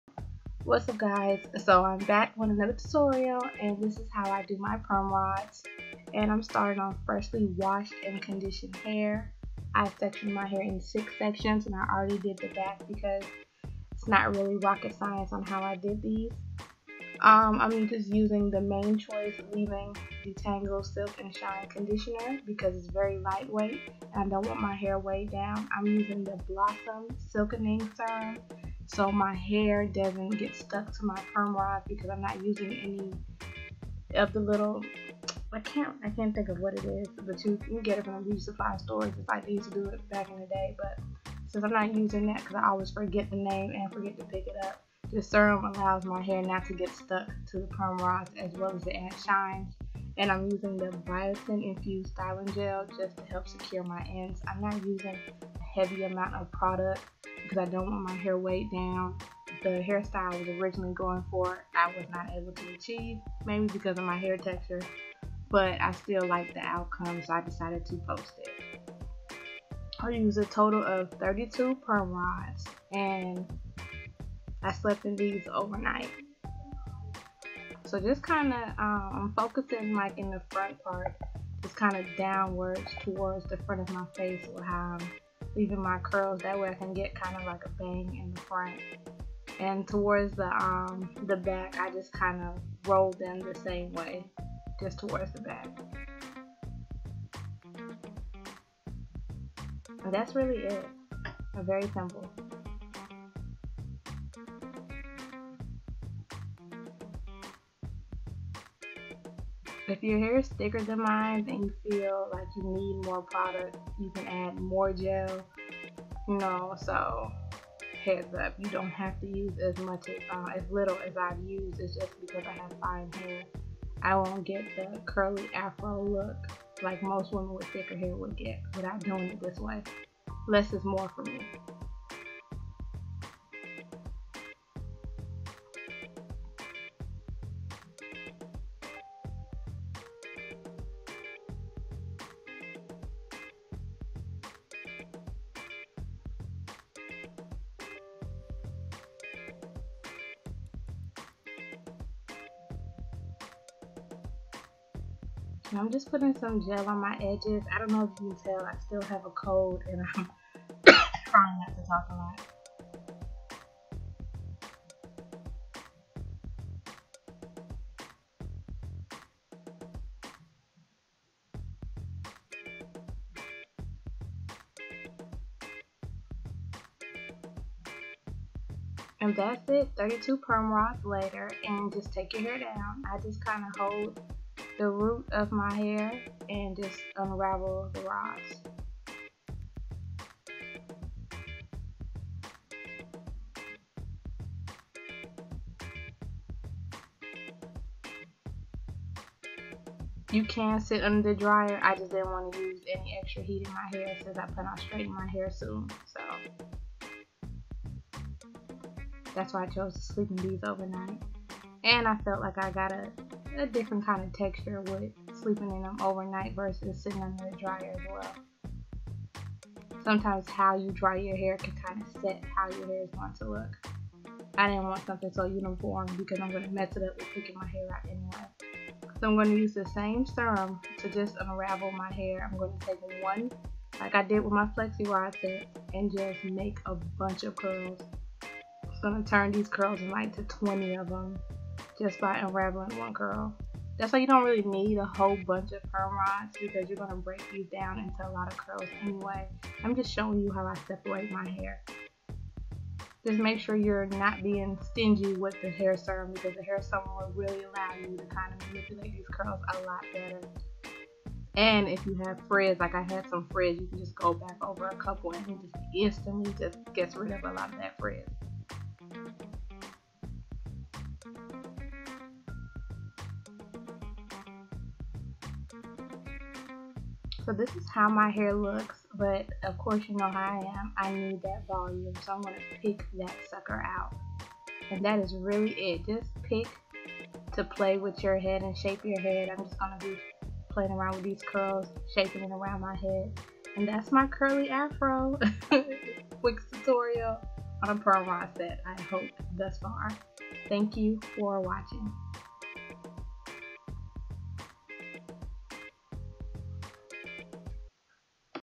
Perm Rod Tutorial Using The Mane Choice/ Curly Fro/heat less curls
Products Used: The Mane Choice
-Silk & Shine Leave-In Conditioner(Give it to me Straight)
-Blossom Silkening Serum
-Crystal Orchid Biotin Infused Styling Gel

Sally Beauty
Rite Aid
Target